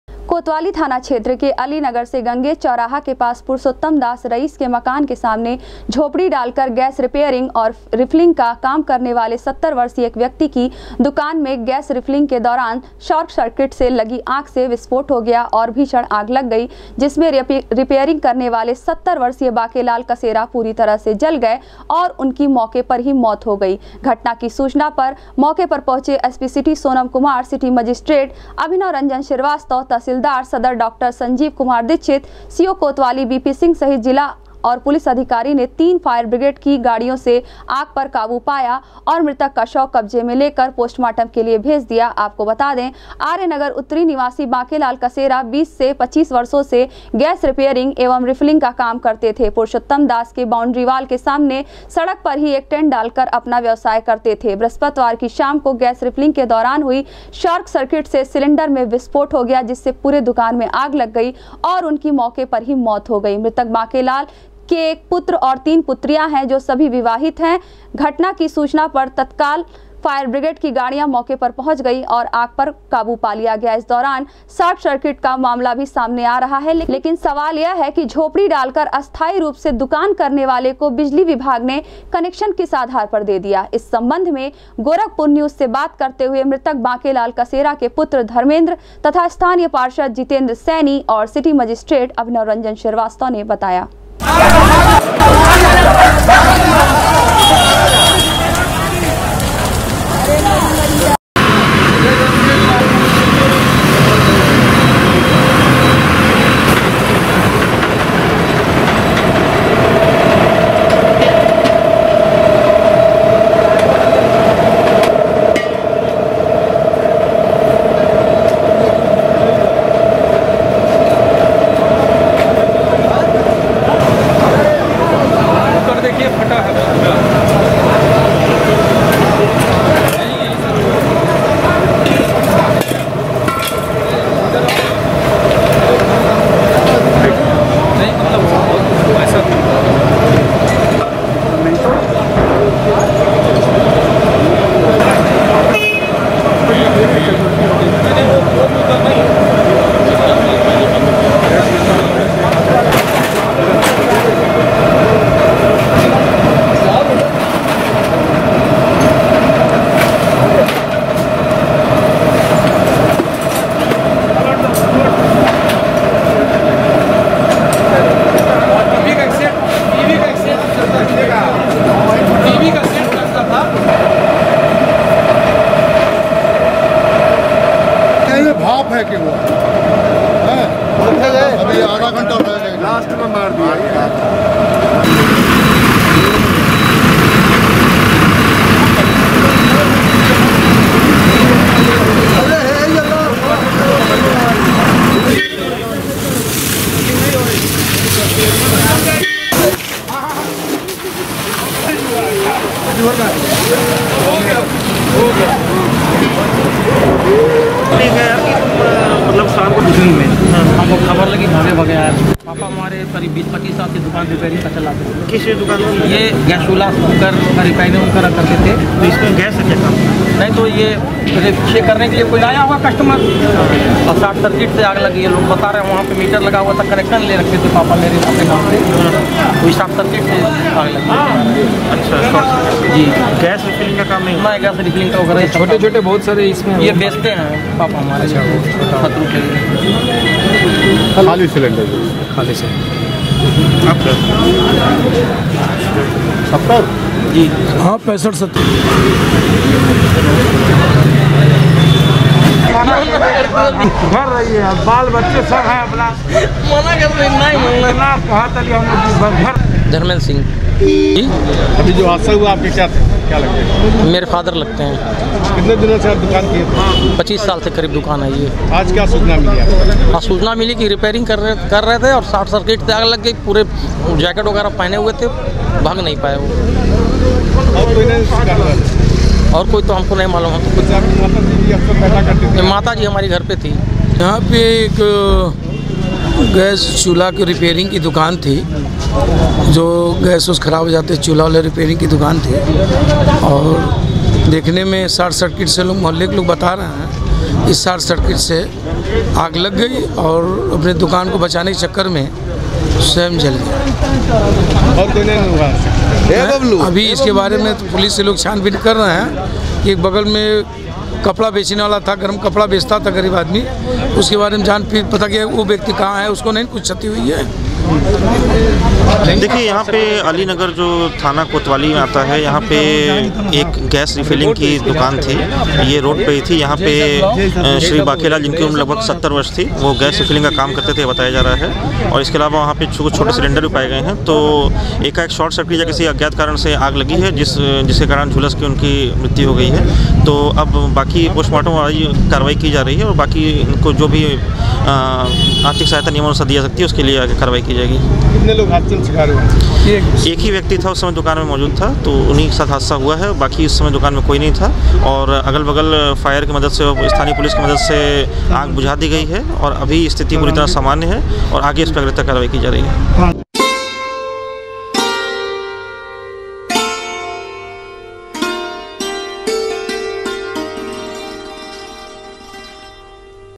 कोतवाली थाना क्षेत्र में गैस रिपेयरिंग और रिफलिंग की दुकान में गैस सिलेंडर के लीक होने के बाद शार्ट
कोतवाली थाना क्षेत्र में गैस रिपेयरिंग और रिफलिंग की दुकान में गैस सिलेंडर के लीक होने के बाद शार्ट सर्किट से लगी आग, एक व्यक्ति की हुई मौत